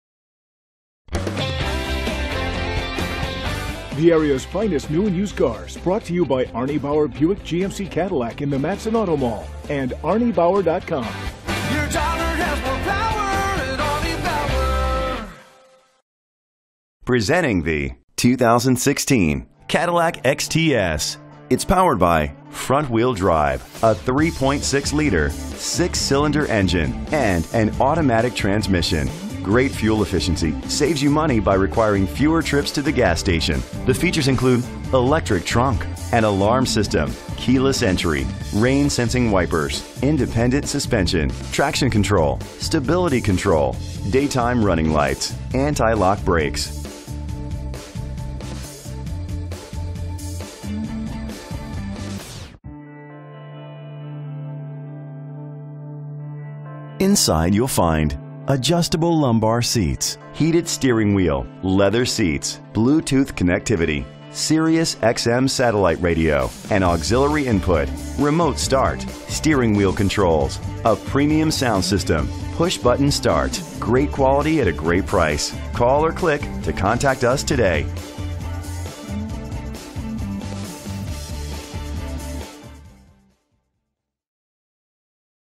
2016 Cadillac XTS C60071 - Matteson IL
Year: 2016
Make: Cadillac
Model: XTS
Trim: Luxury
Engine: 3.6 liter 6 Cylindercylinder FWD
Transmission: Automatic
Color: Black
Mileage: 2
Address:
5525 Miller Circle Drive
Matteson, IL 60443
VIN:2G61M5S37G9122080